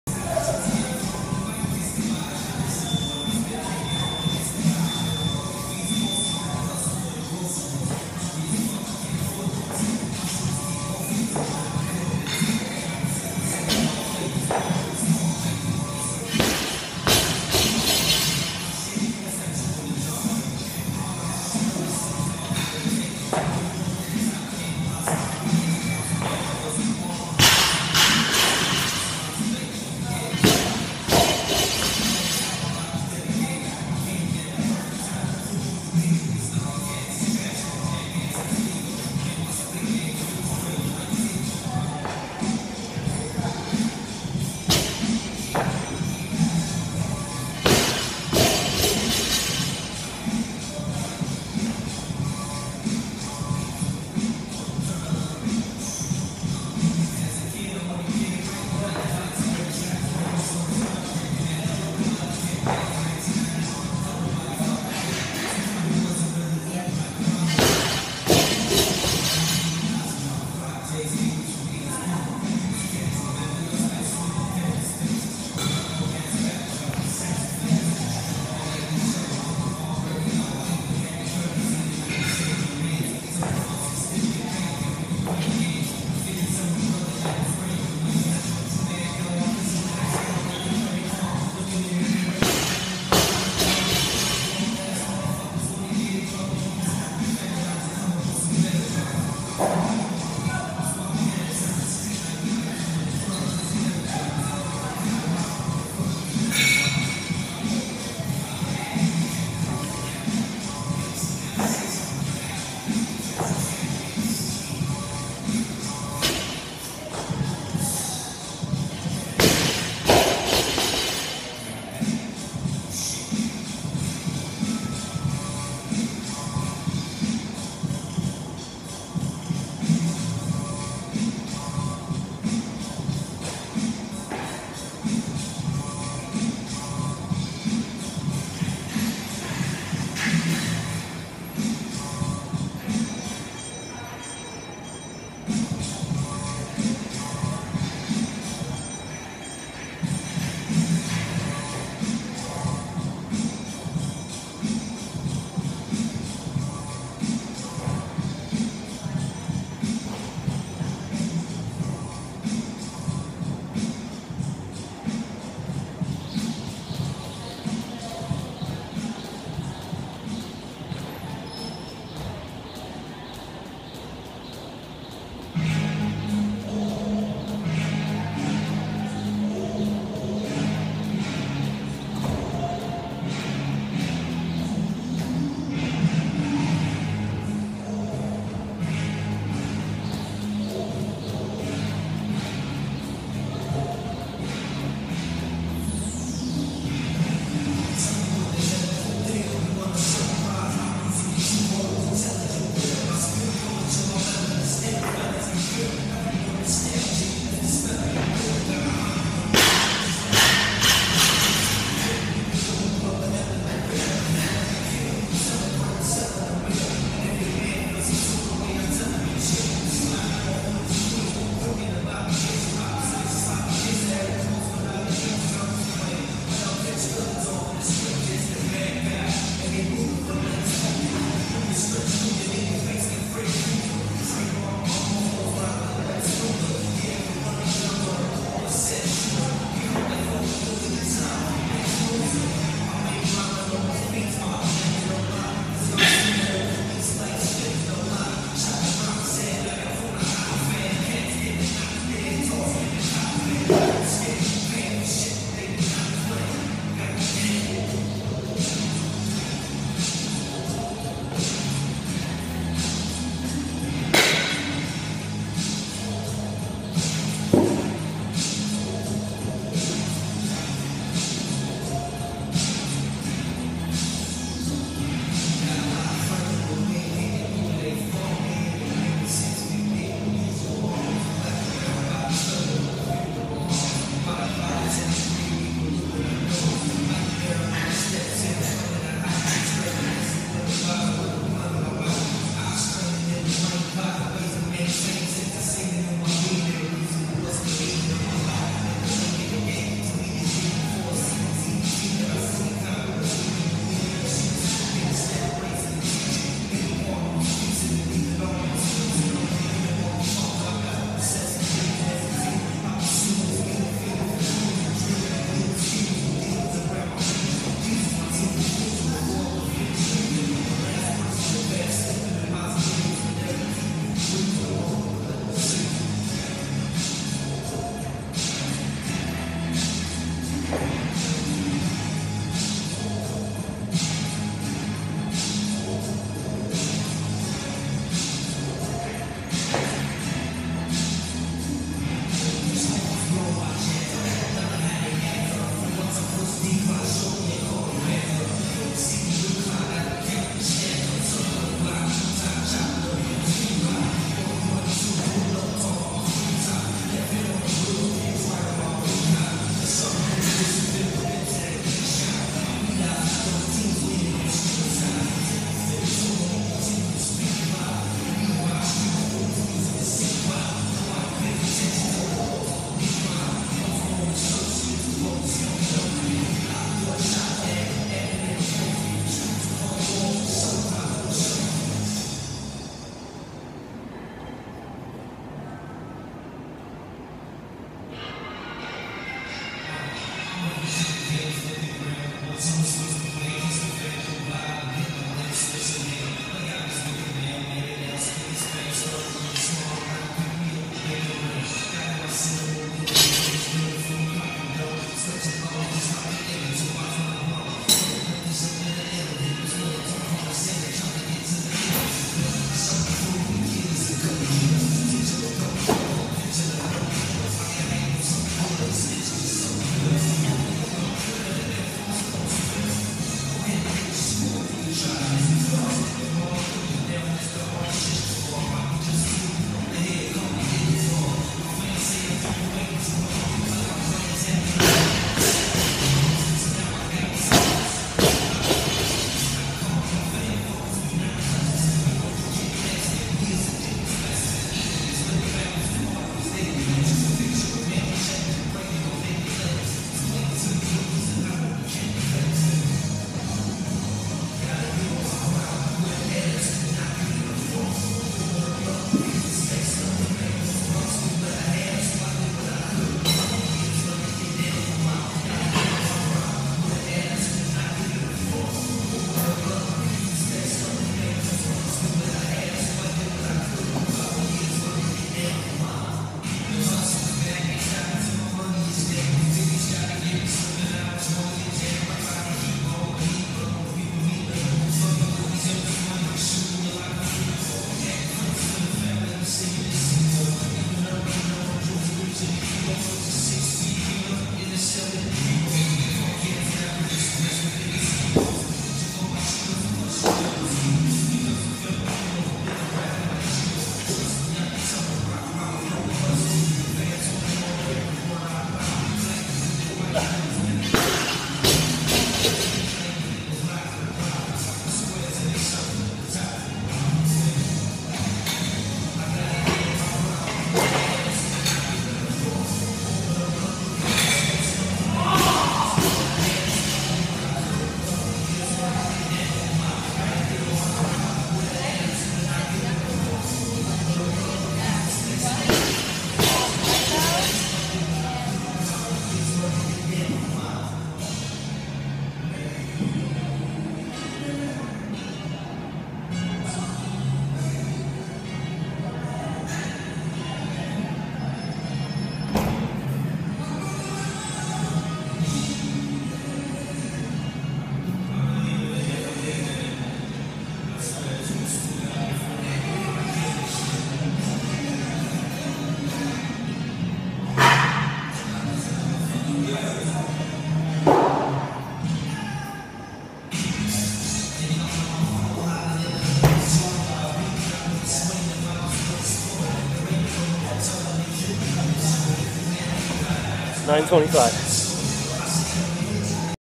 Bellotti's Workout
5 rds (did 6, lost count)
1 power clean
1 hang power clean
1 hang clean
1 clean

Then
3 rds
10 chest 2 bar pullups
50 double unders

Then
2 rds
10 hang power cleans
10 handstand push-ups

9:25
(175# on the bar for all cleans)